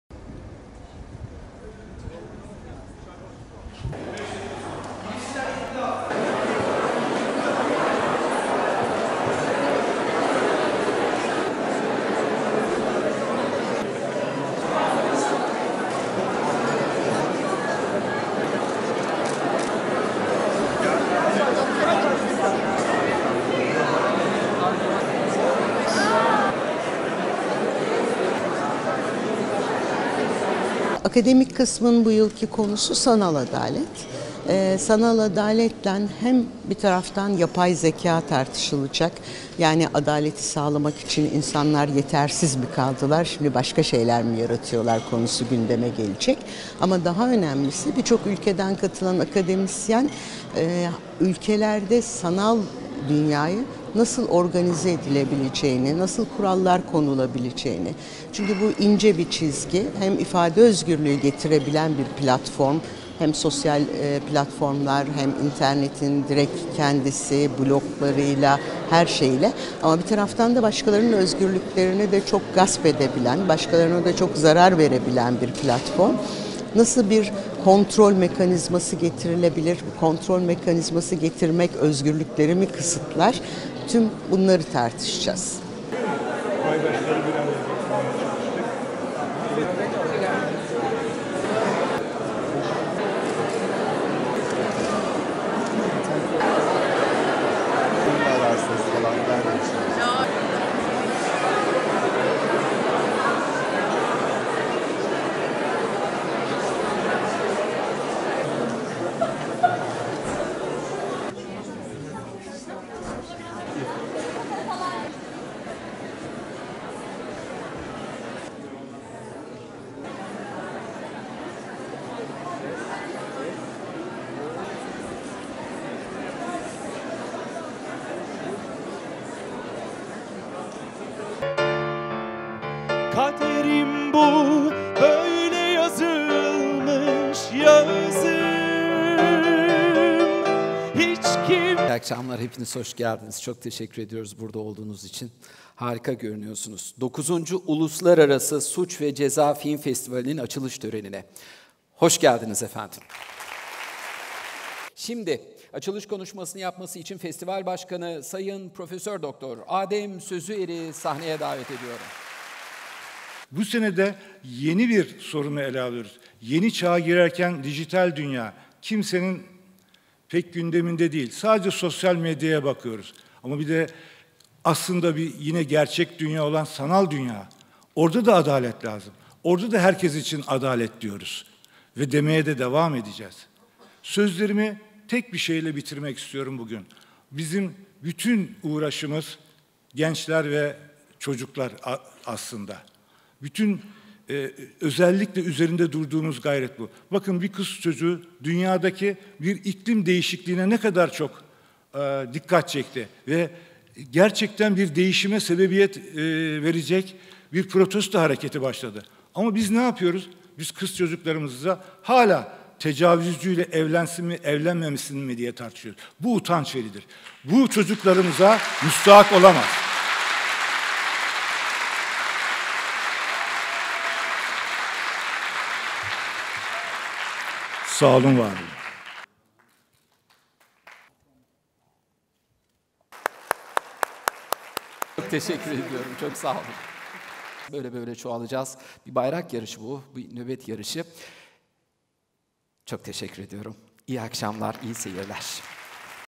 9. Uluslararası Suç ve Ceza Film Festivali CRR'de düzenlenen törenle başladı
Adalet temasıyla her yıl düzenlenen "9. Uluslararası Suç ve Ceza Film Festivali" Cemal Reşit Rey'de gerçekleşen açılış töreniyle başladı. Sinema ve sanat dünyasından ünlü isimlerin katılımıyla yapılan açılışı Yekta Kopan sundu.